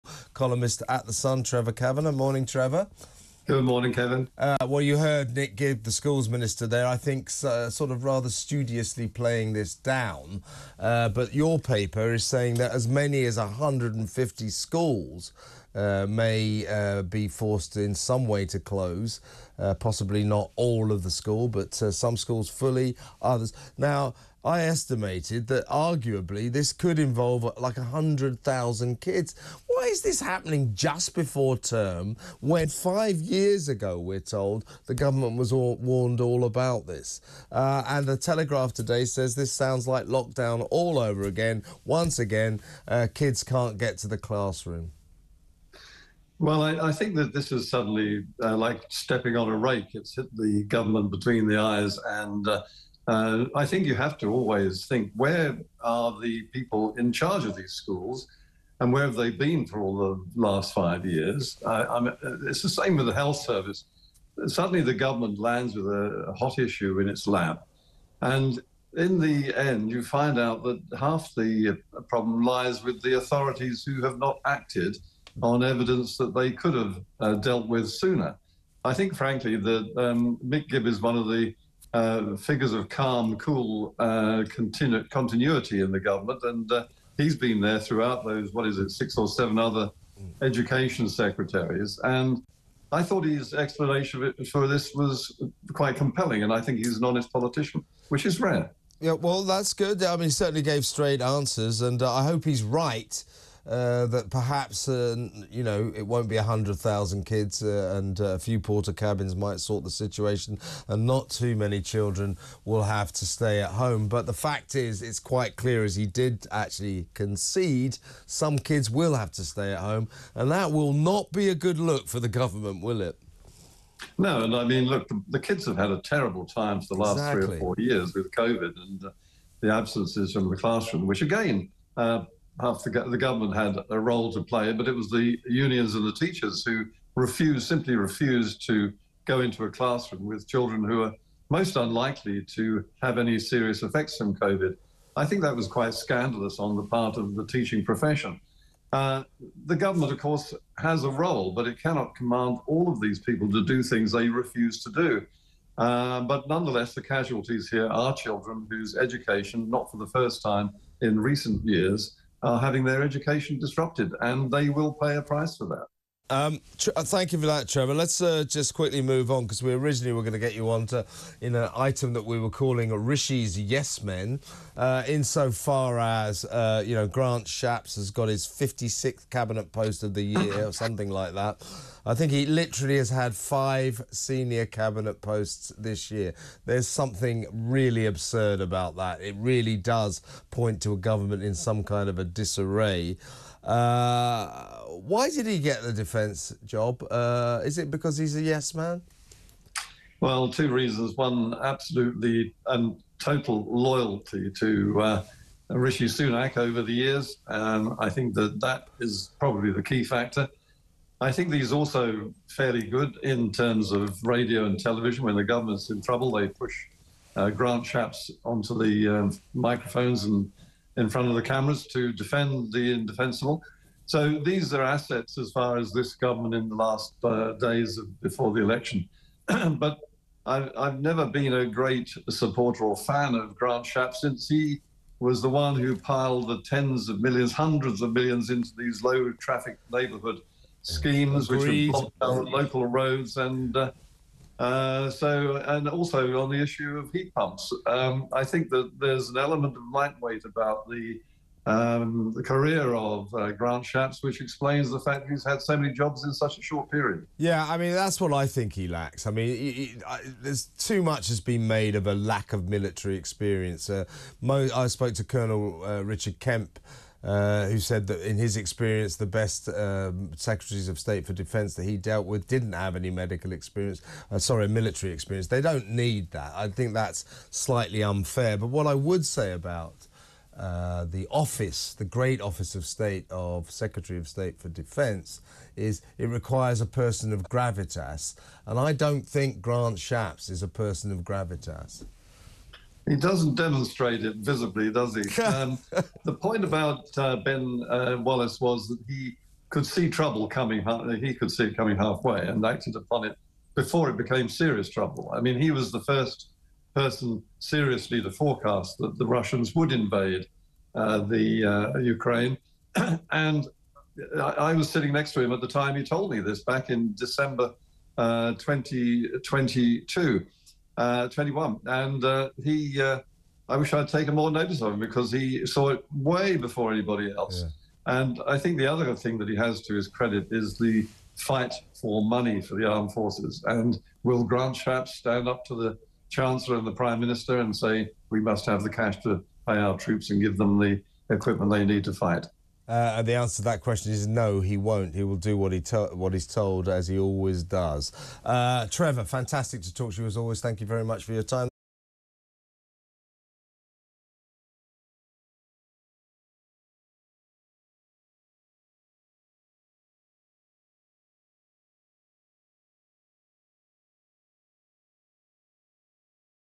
“It’s Hit The Government In The Face!” | Schools To Be Shut Due To Collapse Prone Concrete
Some schools could be told they need to shut classrooms because they are fitted with a concrete that could suddenly collapse, the Schools Minister has admitted.

Nick Gibb insisted that the Government will pay for the costs of temporary accommodation after official guidance suggested schools will have to cover the emergency measures.

The Department for Education has told 104 schools and colleges to partially or fully close buildings just as pupils prepare to return after the summer holidays.

The Sun’s Trevor Kavanagh tells TalkTV’s Kevin O’Sullivan that some schools closing due to concrete prone to collapse is like the Government “stepping on a rake.”

"It's hit them between the eyes. Where are the people in charge of these schools and where have they been for the last five years?"